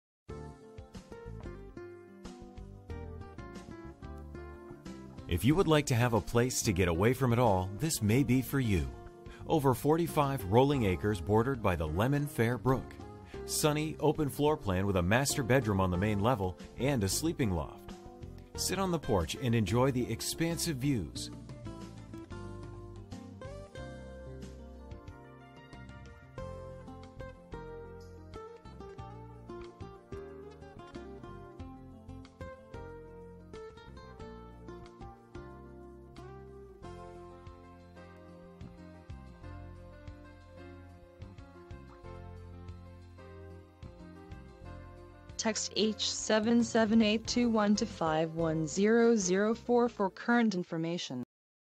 Approx 80 Old Sawmill Road Orwell VT
If you would like to have a place to get away from it all this may be for you  Over forty five rolling acres bordered by the Lemon Fair Brook Sunny open floor plan with a master bedroom on the main level and a sleeping loft with roo Sit on the porch and enjoy the expansive views or on those chilly spring and fall nights y which was built with stone from the property or a gas stove for convenience Solar powered

Video uploaded on Thu, 21 Nov 2013 02:00:07 -0600